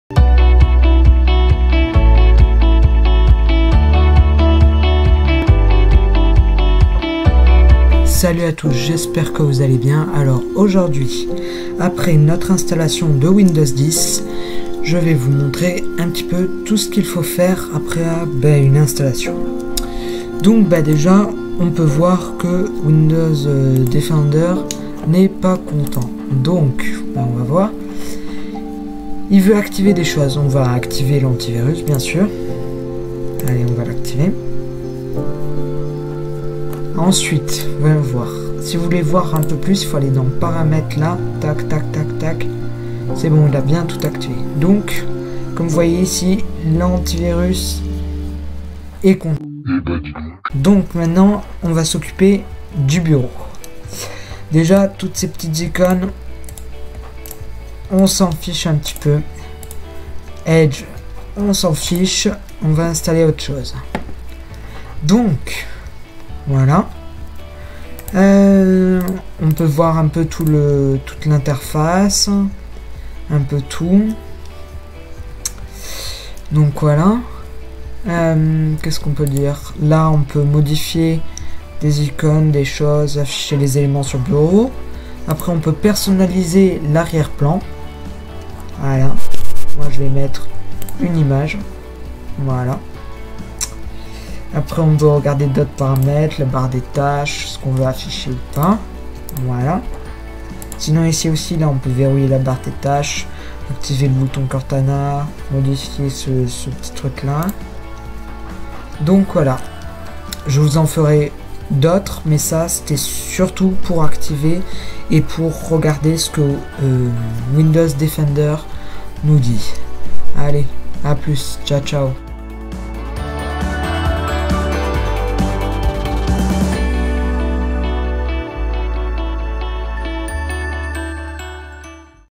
S1 - E2 : Windows defender + interface de base de Windows
Dans ce second épisode, je vous montre comment activer Windows defender et comment le vérifier.
Puis dans un second temps, on va regarder l’interface de base de Windows :
- Les icones ;
- Le fond d'écran ;
- La barre des tâches ;
- Le menu démarrer.
- personnalisation